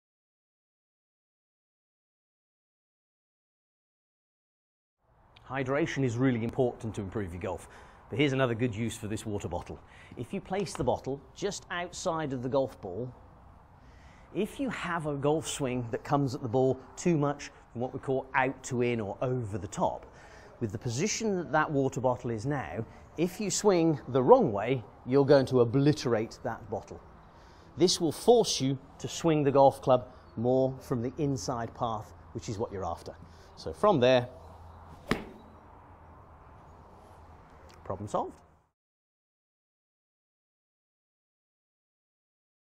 Stop Swinging From The Outside - Jon Woodroffe
Jon Woodroffe demonstrates how to stop from swinging 'out to in' or 'over the top' by using a water bottle. This will correct your swing plane.